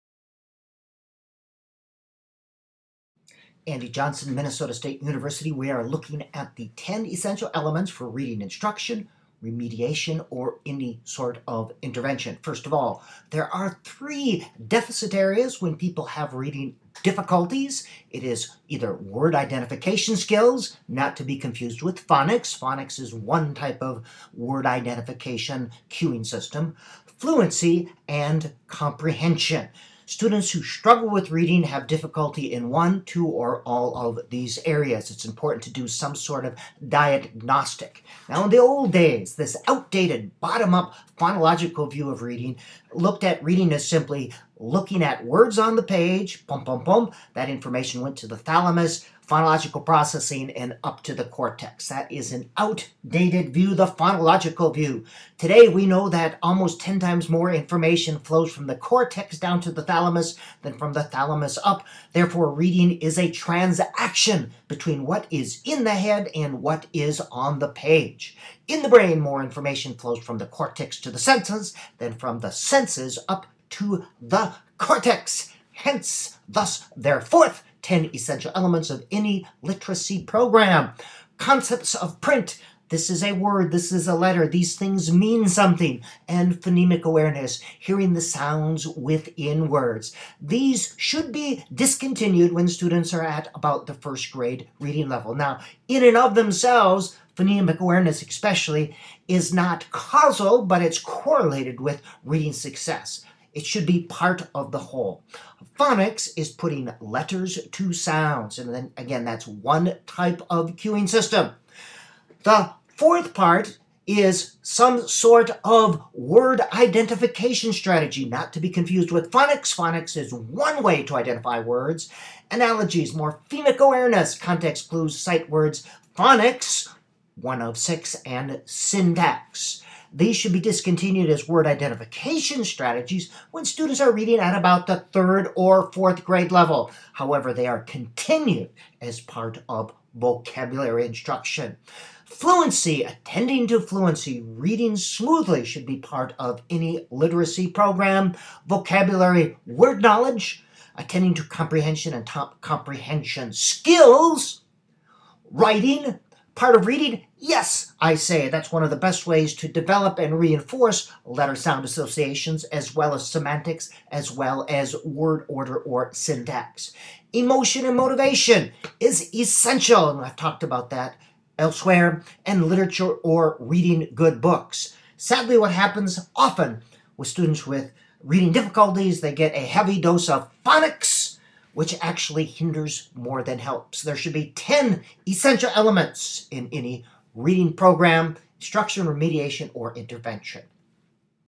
Reading Instruction: 10 Essential Elements
This video mini-lecture describes the 10 essential elements that should be a part of any reading program.  Dr. Andy Johnson, Reading Specialist,